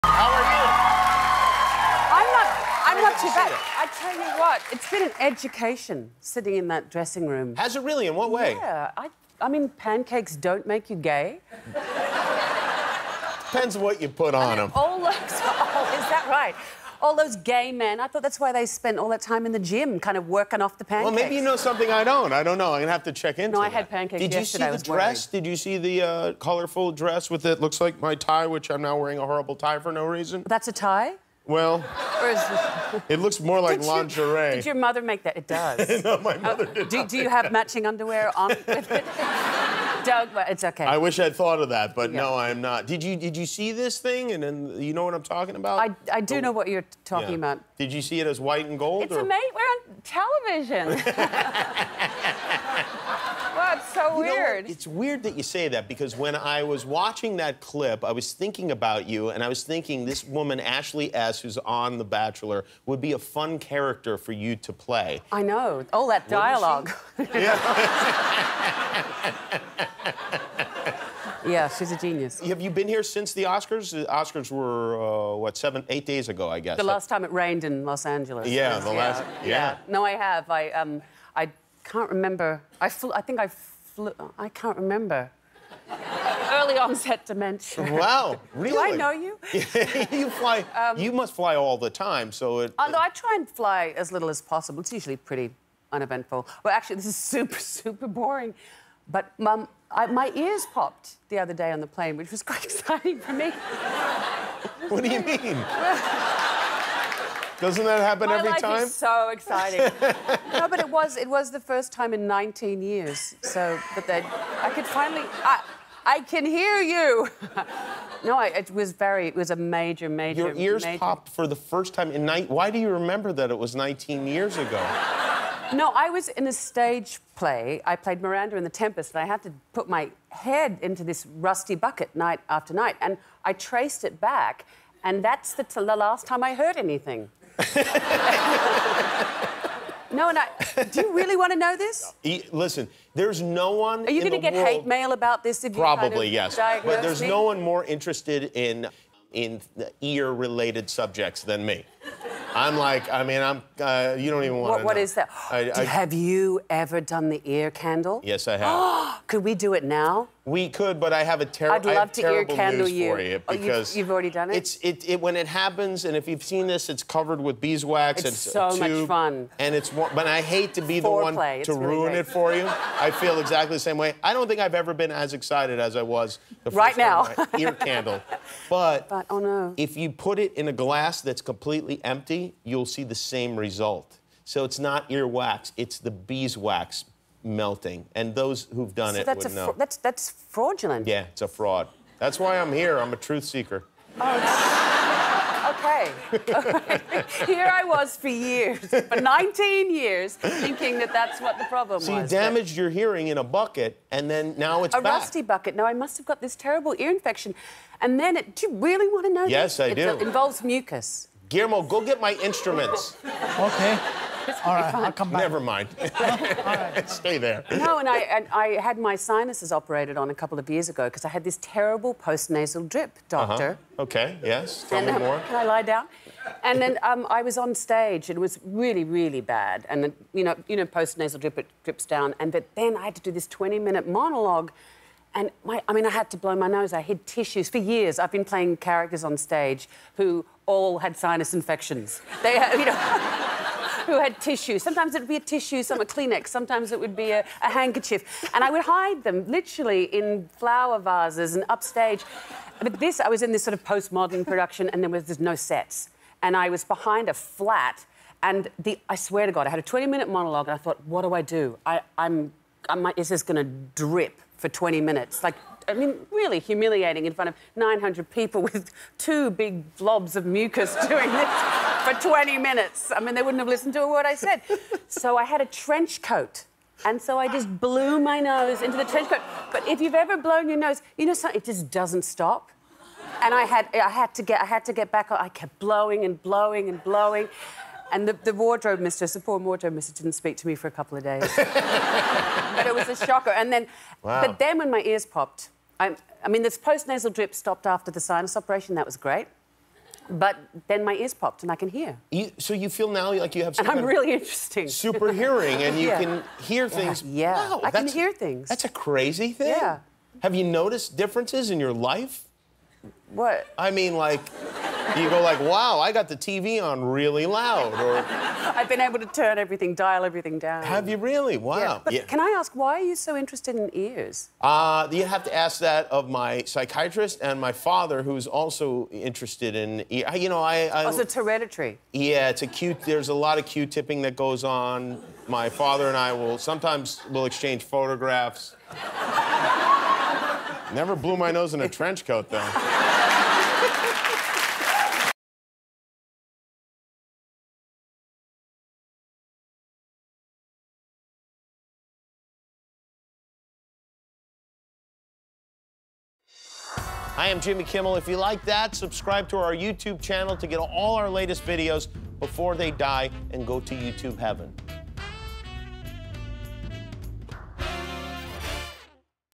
Cate Blanchett’s Ears Popped After 19 Years
Cate talks about her ears popping for the first time in 19 years, prompting her and Jimmy talk at length about their fascination with ears.